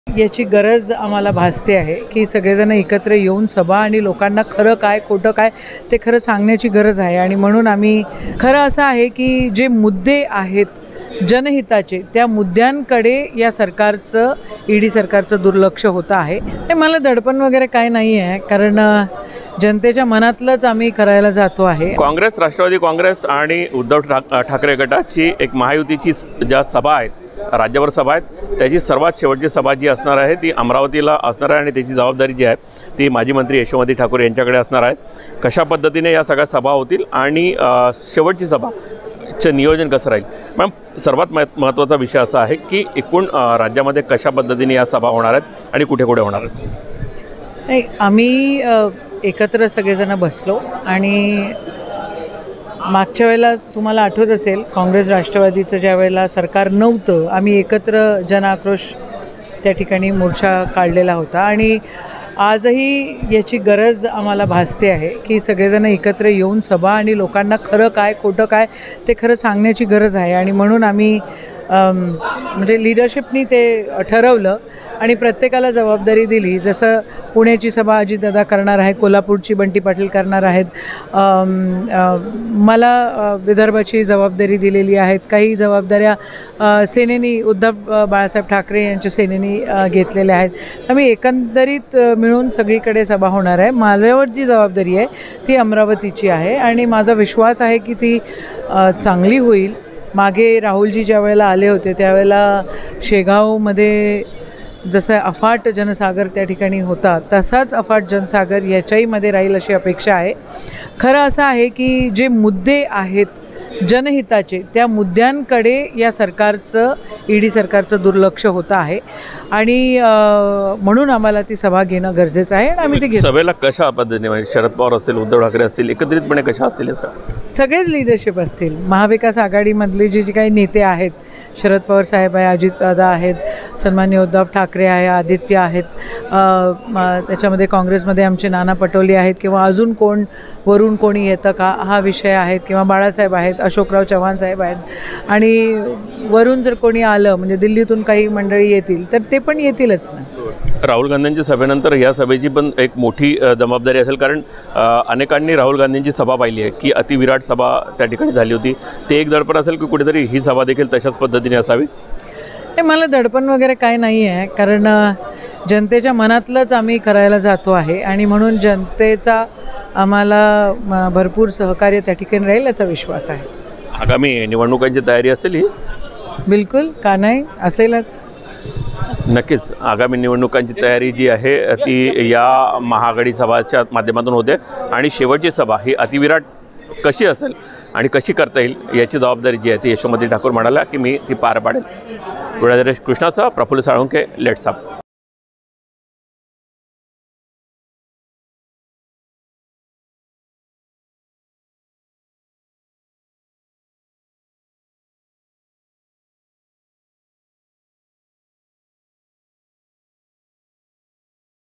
Yashomati Thakur | सभेचे नियोजन, उपस्थिती आणि तयारी... ठाकूर Exclusive | LetsUpp Marathi
महाविकास आघाडीच्या वतीने राज्यभर सभा होणार आहे. यातील शेवटची सभा अमरावतीमध्ये होणार असून त्याची जबाबदारी माजी मंत्री यशोमती ठाकूर यांच्याकडे आहे. या पार्श्वभूमीवर लेट्सअपचे विशेष प्रतिनिधी प्रफुल्ल साळुंखे यांनी ठाकूर यांच्याशी संवाद साधला होता.